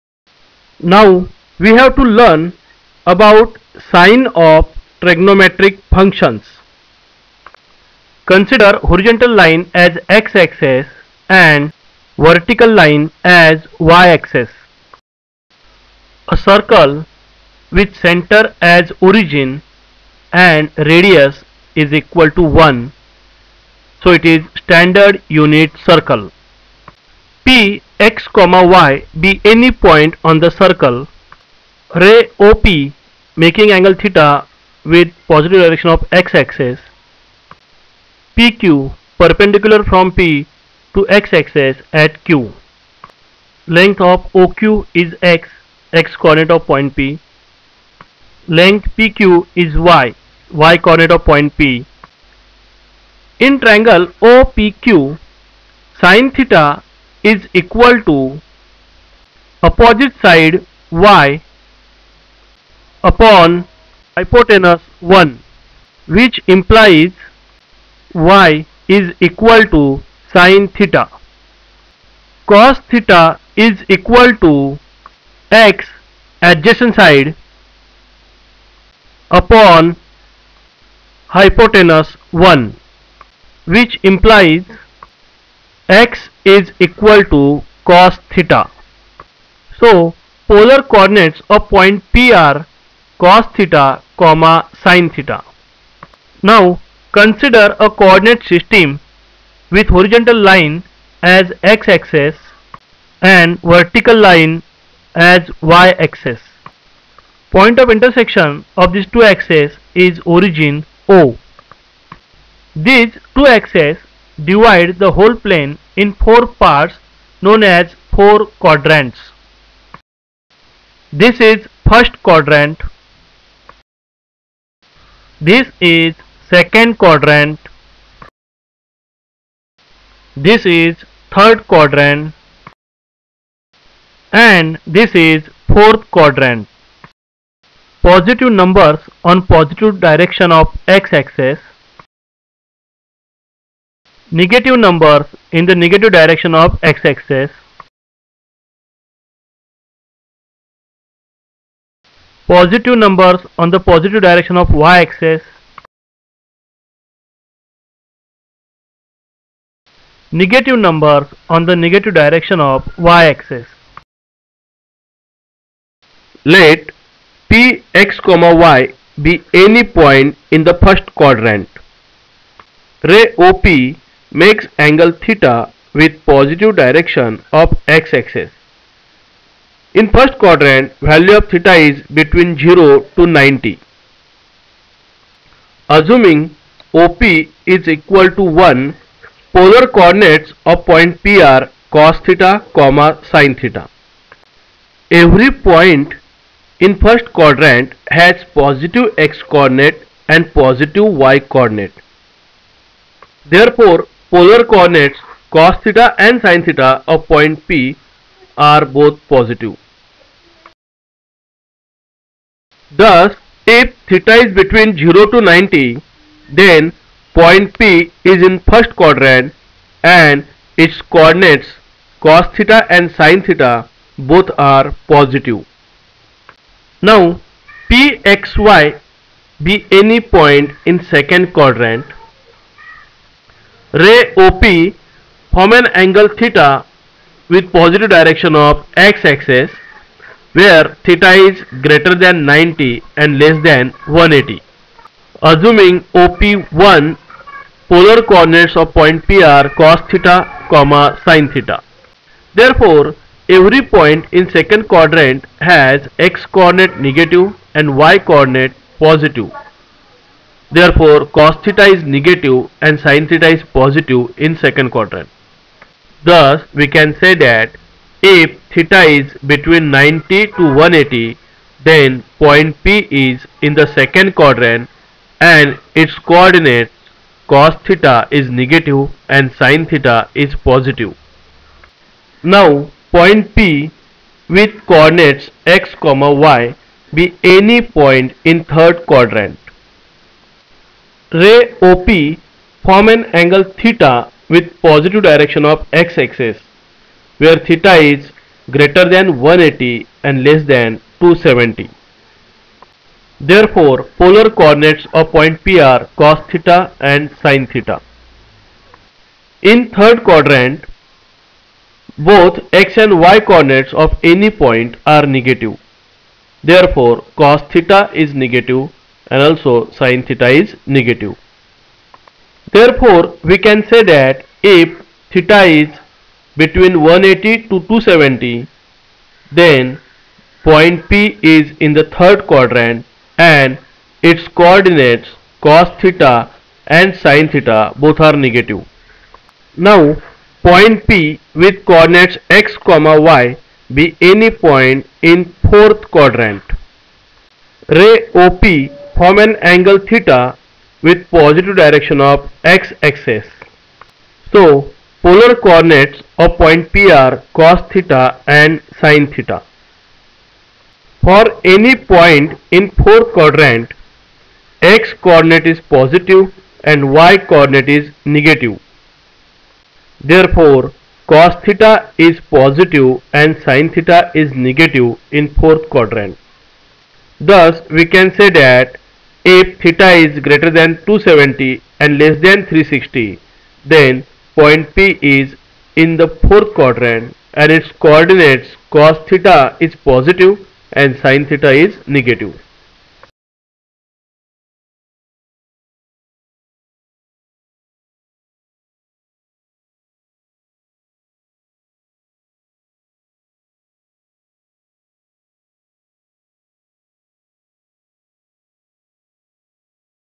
SIGN OF TRIGONOMETRIC FUNCTIONS according to quadrants or angles: CLASS 11 : TRIGONOMETRY
Basic Trigonometry for begginers
2.1.1 Trigonometric functions with the help of a circle:
Useful for class 9, class 10, class11, class12 and diploma engineering
CBSE, ICSE, SSC, HSC, IGCSE, IB AND ALL COMPETITIVE EXAMS